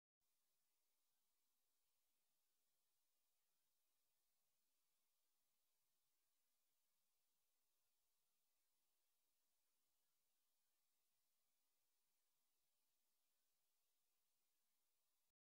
Turtle vs. Shark
Second example of a turtle in close proximity to a shark. Observed 40 to 60 miles offshore of New Jersey in the Mid-Atlantic Bight during the summer of 2008. Footage taken with a Teledyne Bethos ROV.